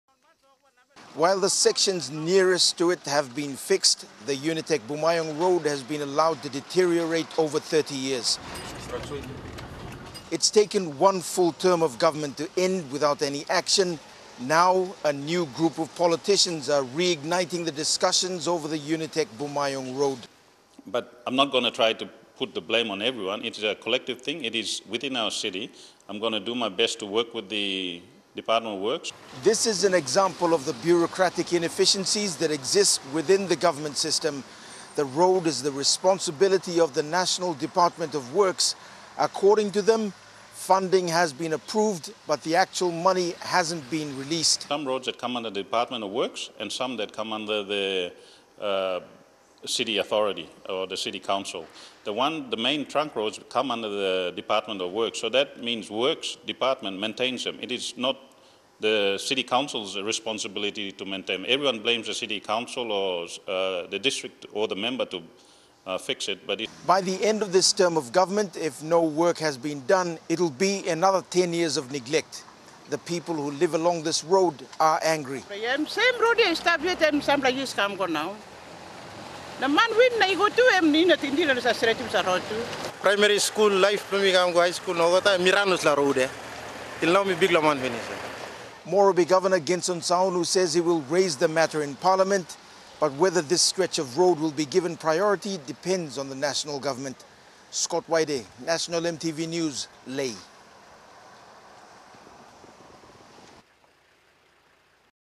Short Stretch of Road Between UNITECH and Bumayong Features Discussions
A short stretch of road between the University of Technology and Bumayong in Lae is featuring again in discussions four months after new Morobe MPs took office.

It is understood the road is the responsibility of the works department and approvals have been given for its rehabilitation.

The road has steadily deteriorated since the 1990s.